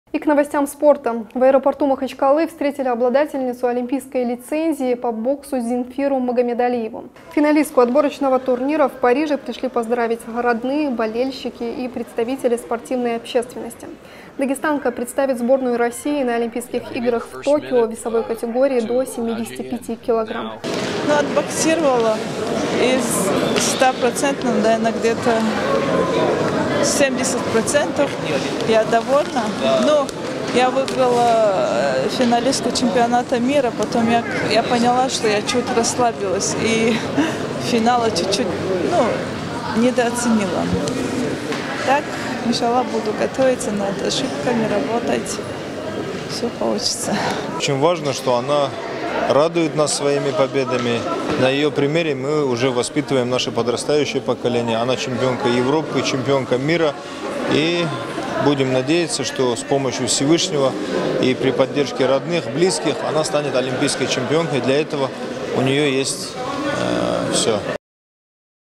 Чемпионку мира по боксу встретили в Махачкале
В аэропорту Махачкалы встретили обладательницу олимпийской лицензии по боксу Зенфиру Магомедалиеву.

Финалистку отборочного турнира в Париже пришли поздравить родные, болельщики и представители спортивной общественности. Дагестанка представит сборную России на Олимпийских играх в Токио в весовой категории до 75 кг.